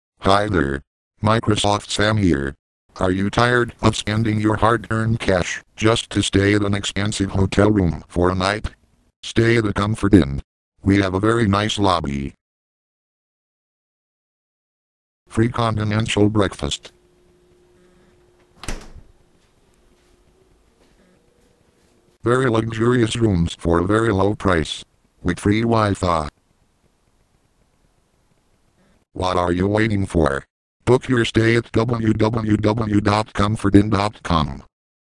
Comfort Inn Commercial
I was bored, so i decided to make a comfort inn commercial